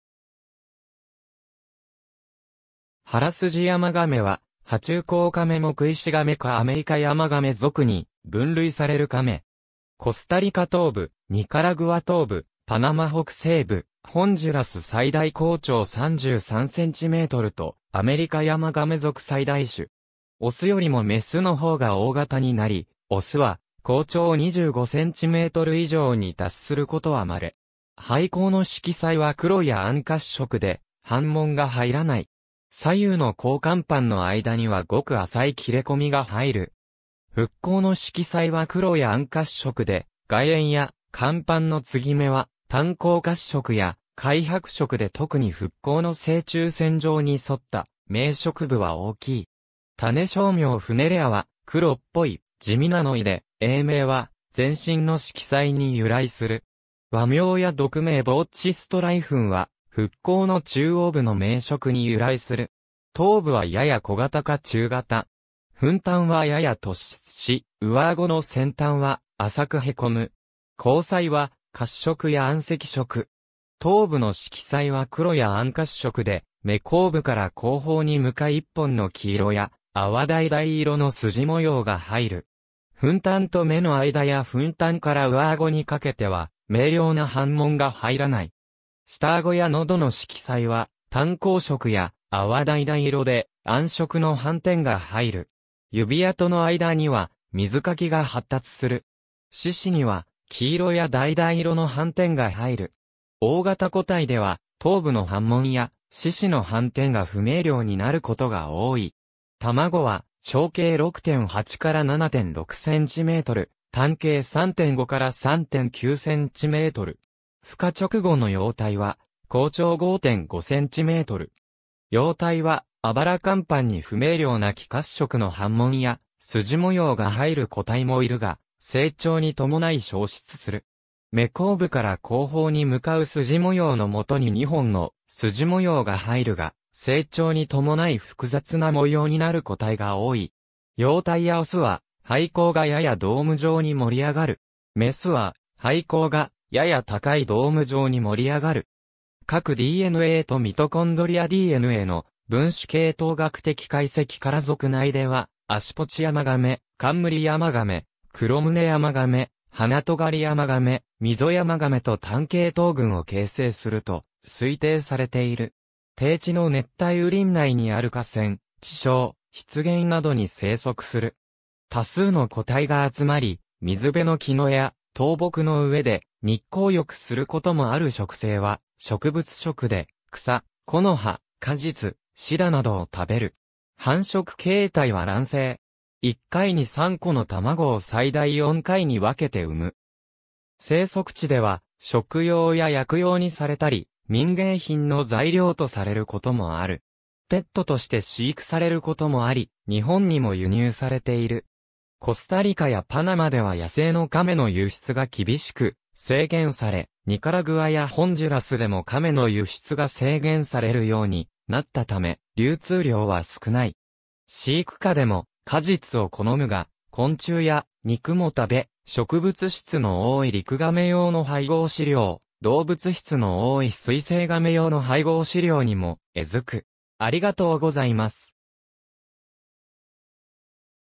ハラスジヤマガメ
ハラスジヤマガメ（Rhinoclemmys funerea）は、爬虫綱カメ目イシガメ科アメリカヤマガメ属に分類されるカメ。
コスタリカ東部、ニカラグア東部、パナマ北西部、ホンジュラス（ココ川水系） 最大甲長33センチメートルとアメリカヤマガメ属最大種。
オスよりもメスの方が大型になり、オスは甲長25センチメートル以上に達することはまれ。
背甲の色彩は黒や暗褐色で、斑紋が入らない。
左右の肛甲板の間にはごく浅い切れ込みが入る。
腹甲の色彩は黒や暗褐色で、外縁や甲板の継ぎ目（シーム）は淡黄褐色や灰白色で特に腹甲の正中線上に沿った明色部は大きい。
種小名funereaは「黒っぽい、地味な」の意で、英名は全身の色彩に由来する。
和名や独名bauchstreifenは、腹甲の中央部の明色に由来する。
頭部はやや小型か中型。
吻端はやや突出し、上顎の先端は浅く凹む。
虹彩は褐色や暗赤色。
頭部の色彩は黒や暗褐色で、眼後部から後方に向かい1本の黄色や淡橙色の筋模様が入る。
吻端と目の間や吻端から上顎にかけては明瞭な斑紋が入らない。
下顎や喉の色彩は淡黄色や淡橙色で、暗色の斑点が入る。
指趾の間には水掻きが発達する。
四肢には黄色や橙色の斑点が入る。
大型個体では頭部の斑紋や四肢の斑点が不明瞭になることが多い。
卵は長径6.8-7.6センチメートル、短径3.5-3.9センチメートル。
孵化直後の幼体は甲長5.5センチメートル。
幼体は肋甲板に不明瞭な黄褐色の斑紋や筋模様が入る個体もいるが、成長に伴い消失する。
眼後部から後方に向かう筋模様の下に2本の筋模様が入るが、成長に伴い複雑な模様になる個体が多い。
幼体やオスは背甲がややドーム状に盛り上がる。
メスは背甲がやや高いドーム状に盛り上がる。
核DNAとミトコンドリアDNAの分子系統学的解析から属内ではアシポチヤマガメ、カンムリヤマガメ、クロムネヤマガメ、ハナトガリヤマガメ、ミゾヤマガメと単系統群を形成すると推定されている。
低地の熱帯雨林内にある河川、池沼、湿原などに生息する。
多数の個体が集まり、水辺の樹上や倒木の上で日光浴することもある 食性は植物食で、草、木の葉、果実、シダなどを食べる。
1回に3個の卵を最大4回に分けて産む。
生息地では食用や薬用にされたり、民芸品の材料とされることもある。
コスタリカやパナマでは野生のカメの輸出が厳しく制限され、ニカラグアやホンジュラスでもカメの輸出が制限されるようになったため、流通量は少ない。
飼育下でも果実を好むが、昆虫や肉も食べ、植物質の多いリクガメ用の配合飼料、動物質の多い水棲ガメ用の配合飼料にも餌付く。